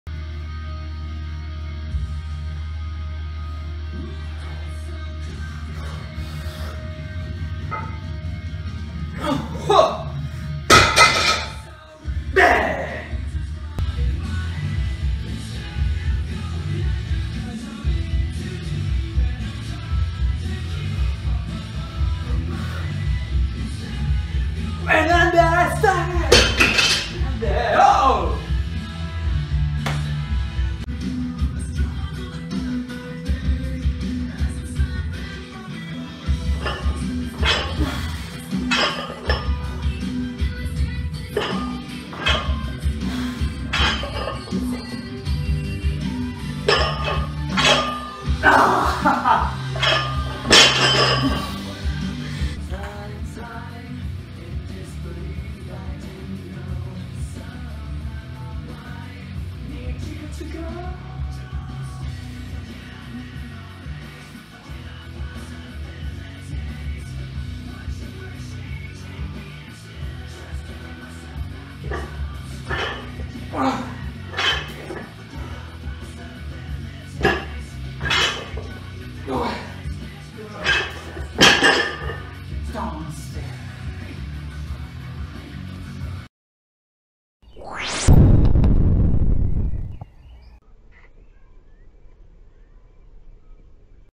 Vertical Jump Training Diary - Powerlifting Improvment
just a little clip of my indoor workout/results in the last 2 months,
soon i will publish my complete routine of these months